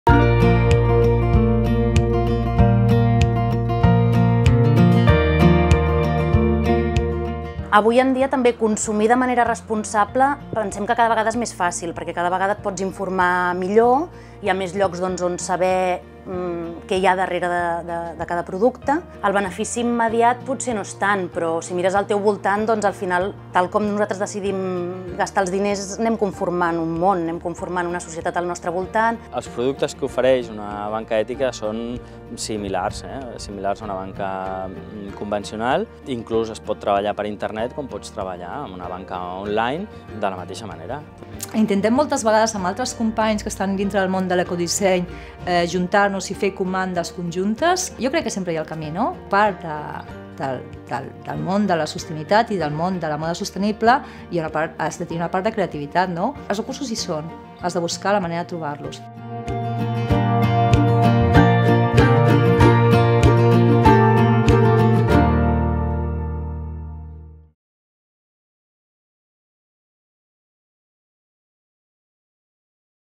Podem ser persones consumidores sempre?
Barcelona aposta per un consum més responsable que contribueixi a aconseguir una societat més justa. A la ciutat existeixen cada cop més facilitats per consumir de manera responsable. Botigues, entitats de finances ètiques, col·lectius... Més informació a